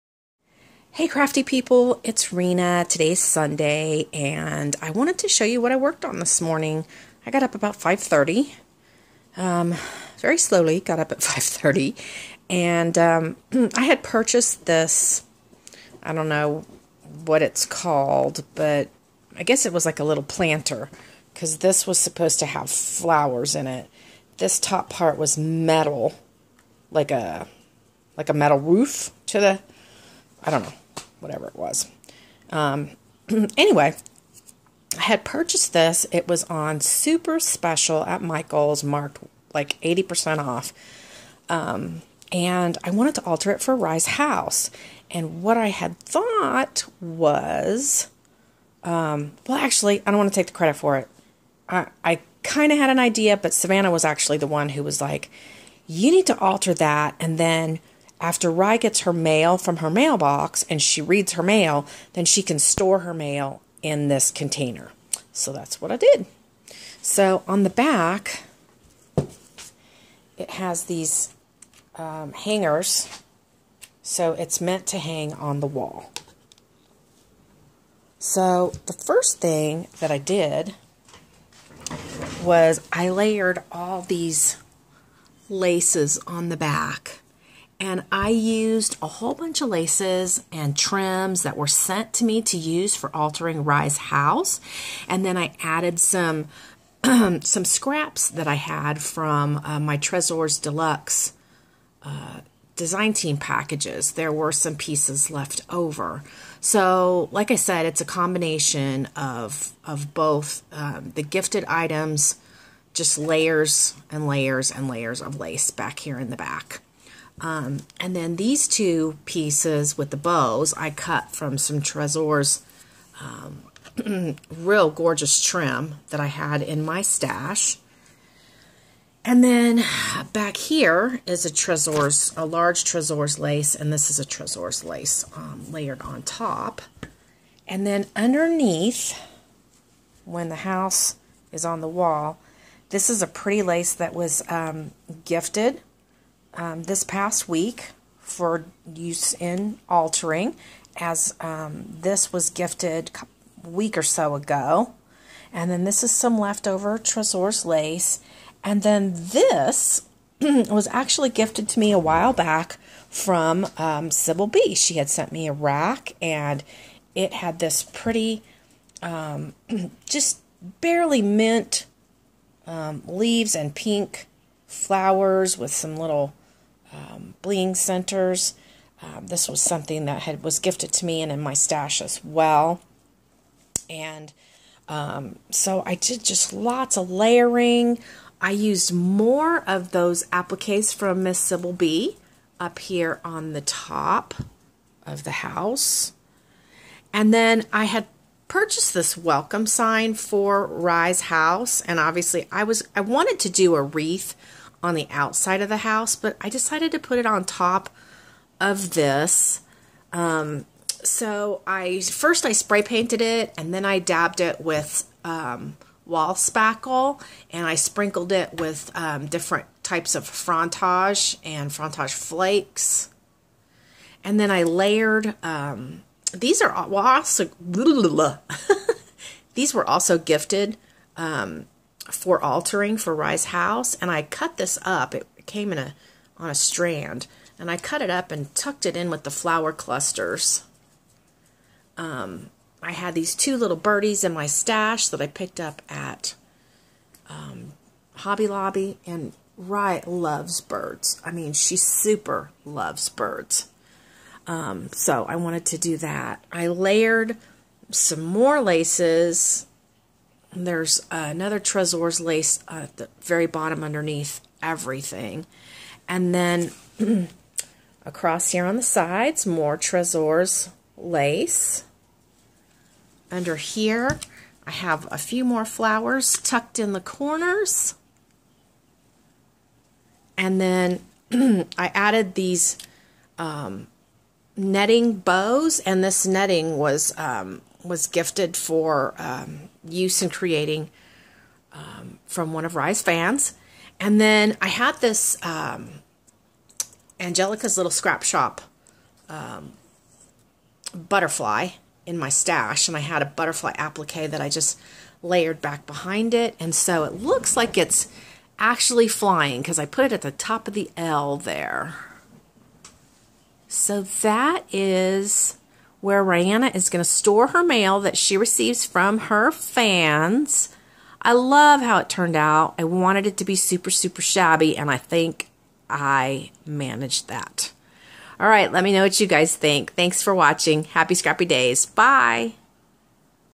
Shabby Chic Altered Storage
I altered this planter for Ryanna's shabby chic playhouse, for her to store her mail from her fans in it!